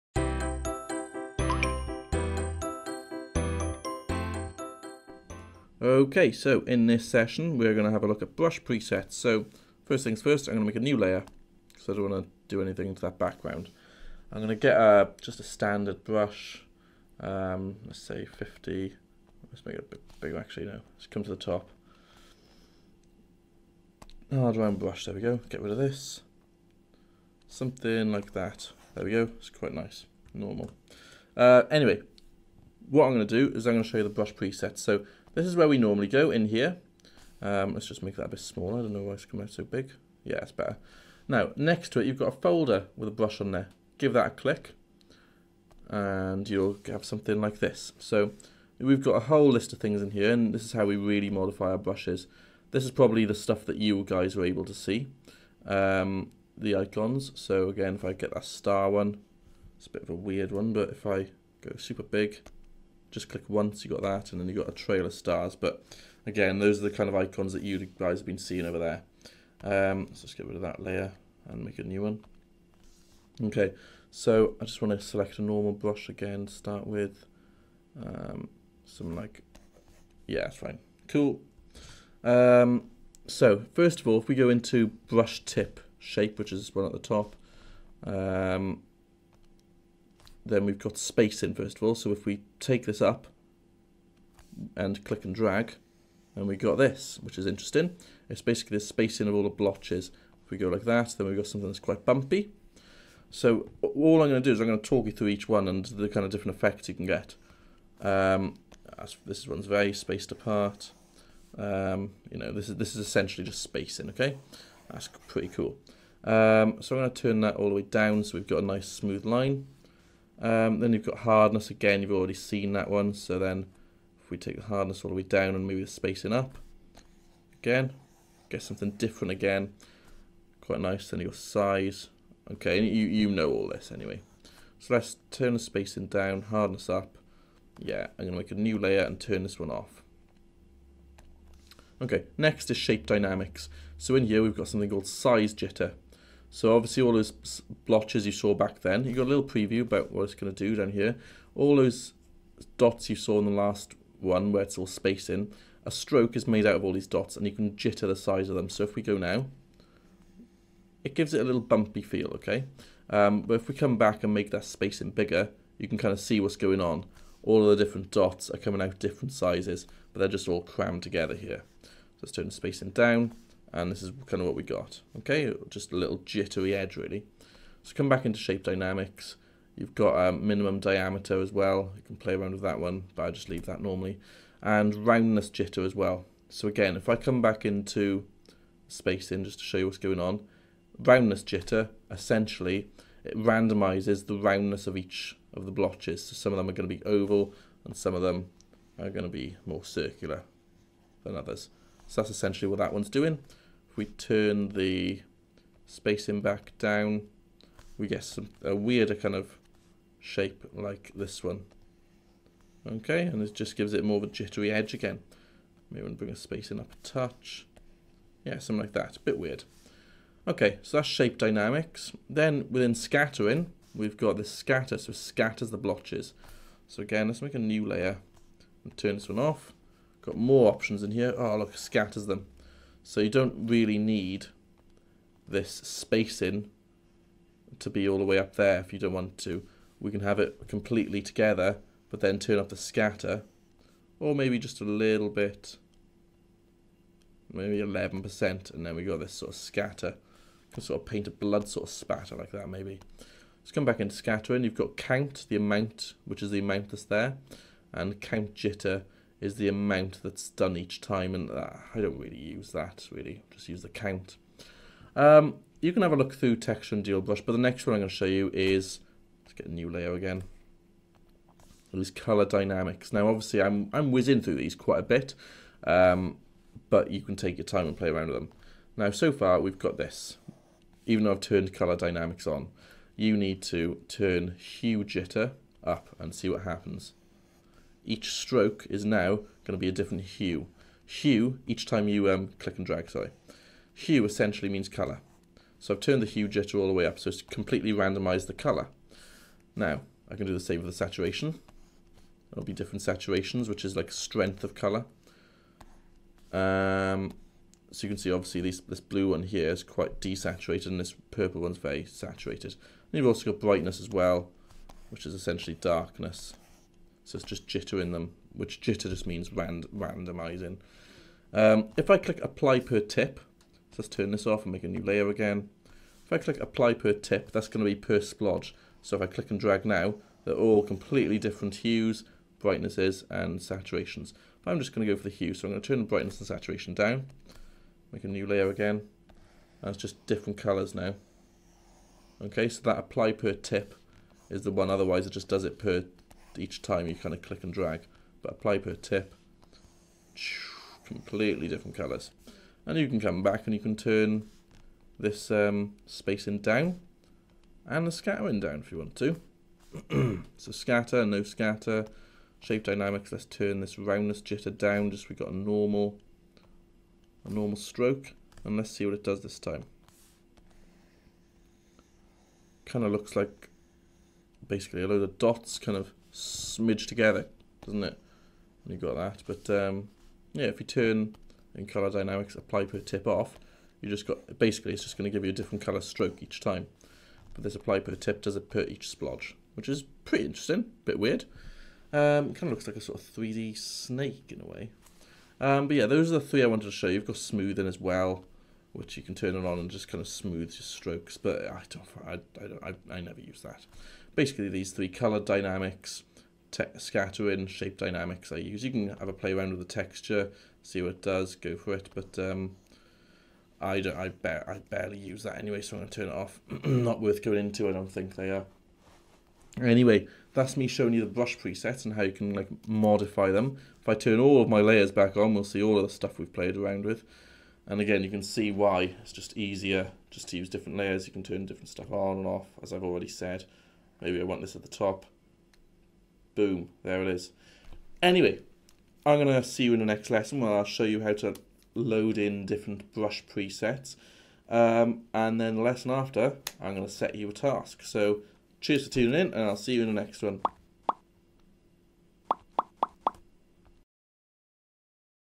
Photoshop for Beginners | How to use brush presets in Photoshop
In this installment of 'Photoshop for Beginners' I wll teach you how to utilise the brush prests in Photoshop to get more control over the way your brush works